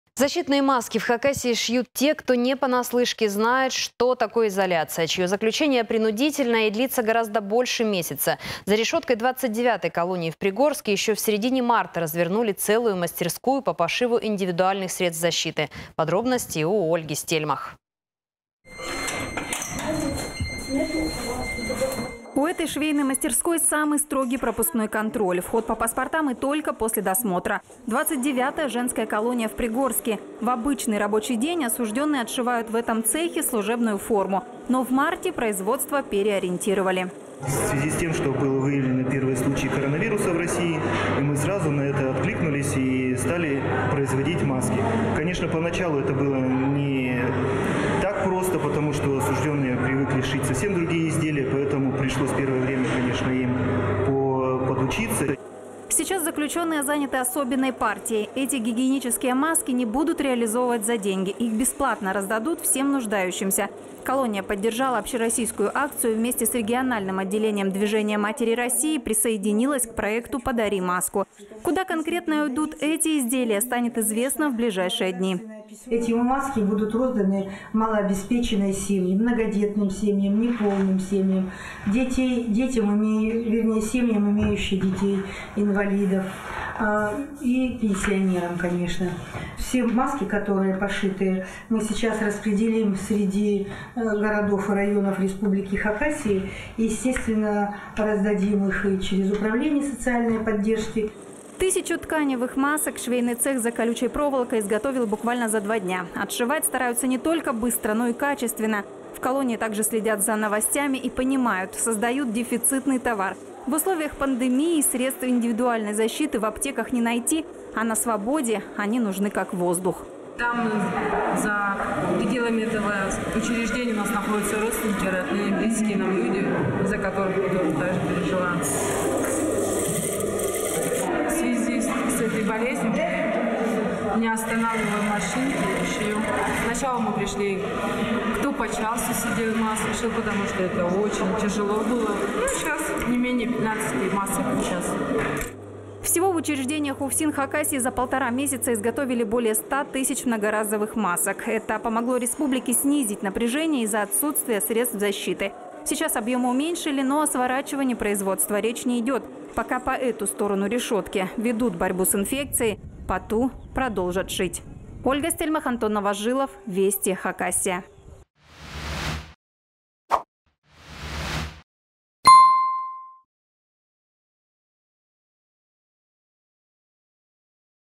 В женской исправительной колонии наладили нужное производство
Защитные маски в Хакасии шьют те, кто не понаслышке знает, что такое изоляция. Чьё заключение принудительное и длится гораздо больше месяца. За решеткой 29-ой колонии в Пригорске еще в середине марта развернули целую мастерскую по пошиву индивидуальных средств защиты.